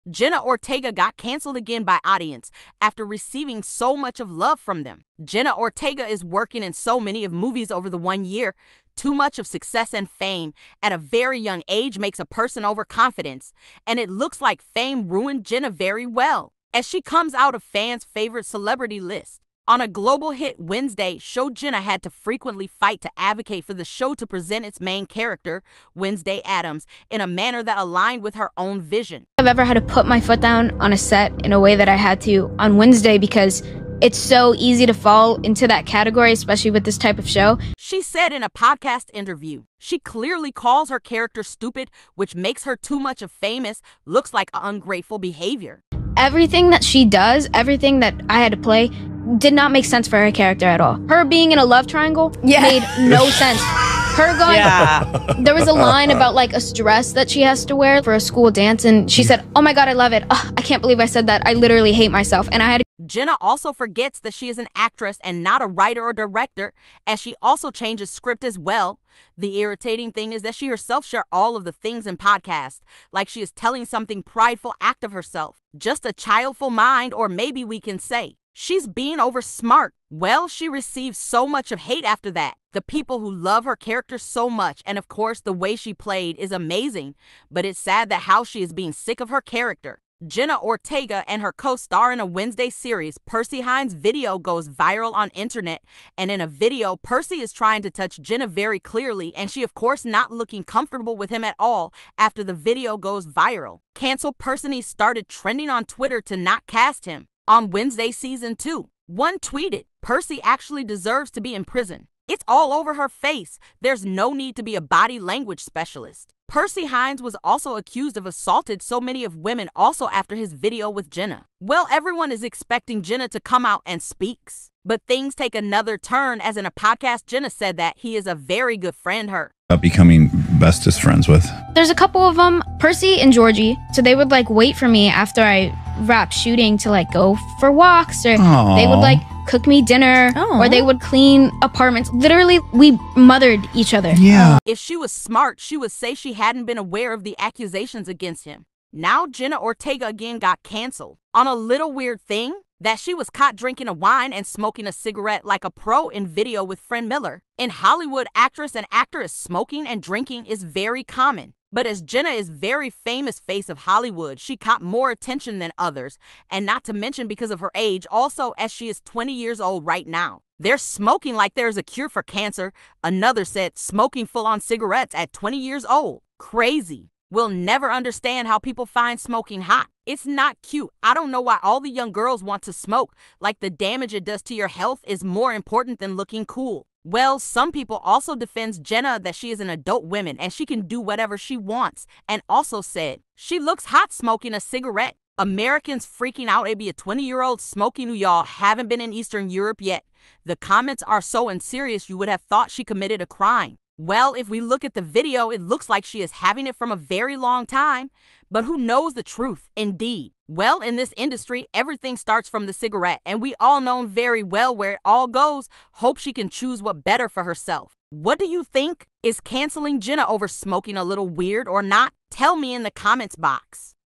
Jenna ortega got CANCELLED Again After her Cigarette Drama! (IS SHE OKAY?)
Jenna Ortega Mom Cancelled her Over Smoking a Cigarette!? (MEAN MOM)